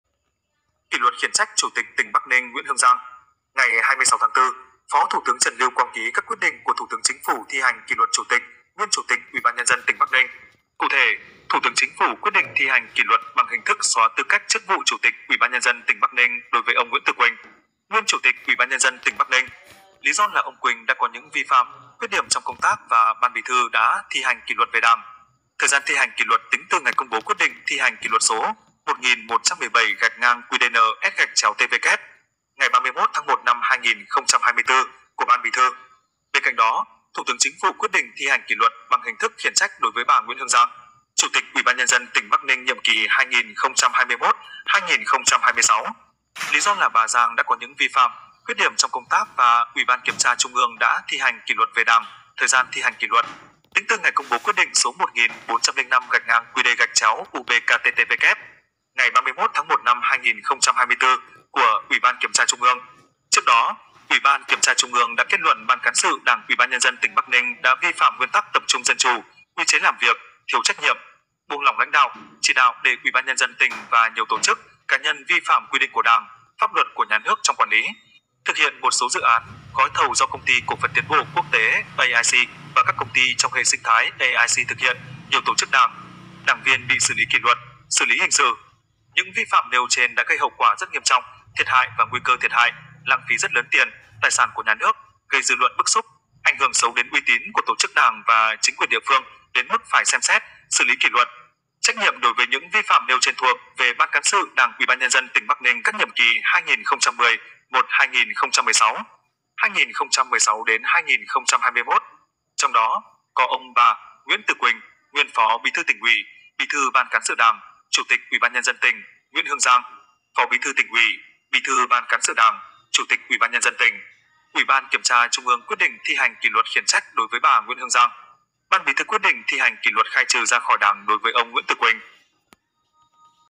Kỷ luật khiển trách Chủ tịch tỉnh Bắc Ninh Nguyễn Hương Giang
Cụ thể, Thủ tướng Chính phủ quyết định thi hành kỷ luật bằng hình thức xóa tư cách chức vụ Chủ tịch UBND tỉnh Bắc Ninh đối với ông Nguyễn Tử Quỳnh, nguyên Chủ tịch UBND tỉnh Bắc Ninh.

Lý do là ông Quỳnh đã có những vi phạm, khuyết điểm trong công tác và Ban Bí thư đã thi hành kỷ luật về đảng; thời gian thi hành kỷ luật tính từ ngày công bố quyết định thi hành kỷ luật số 1117-QĐNS/TW ngày 31/1/2024 của Ban Bí thư.